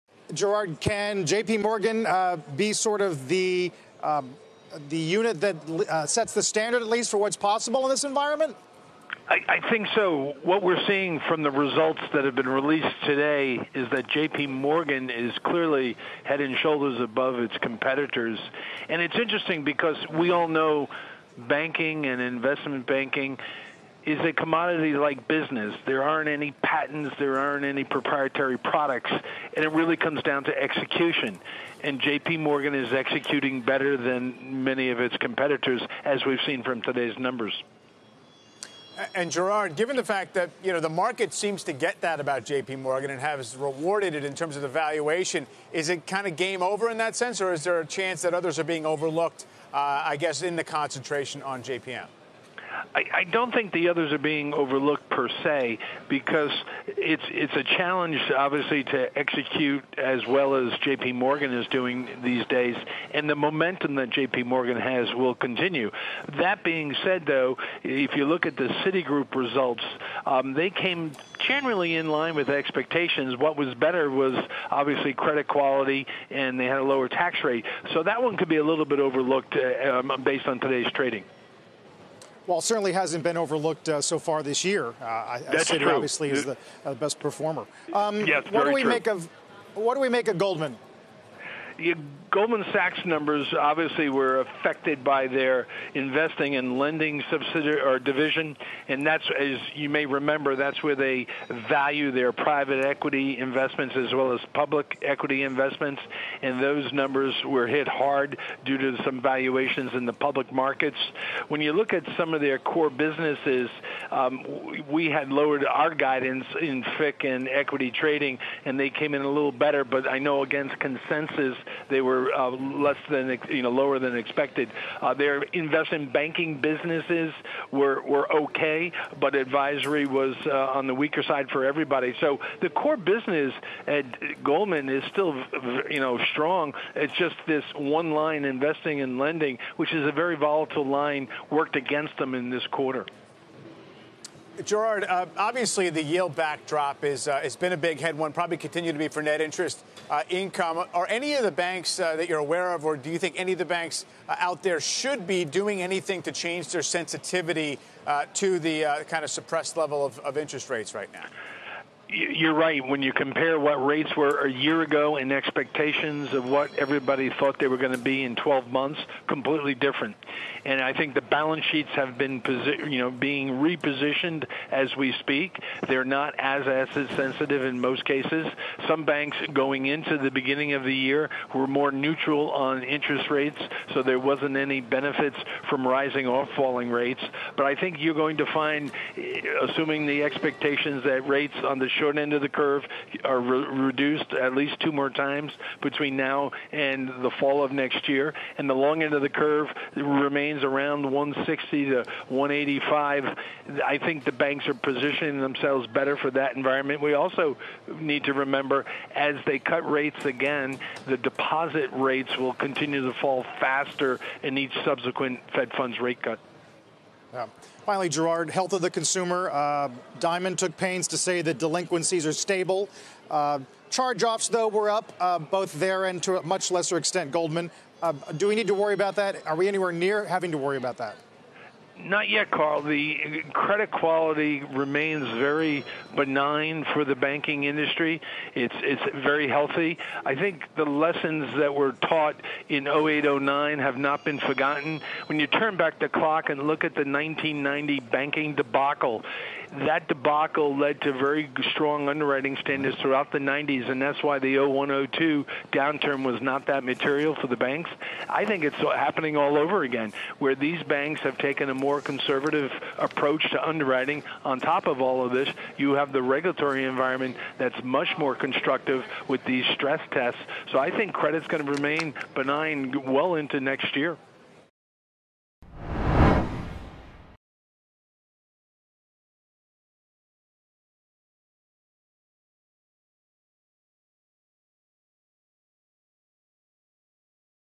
The core business at Goldman Sachs is still strong, says RBC's Gerard Cassidy
Gerard Cassidy, RBC bank analyst, joins "Squawk on the Street" via phone to discuss the major bank stocks after they reported mixed earnings.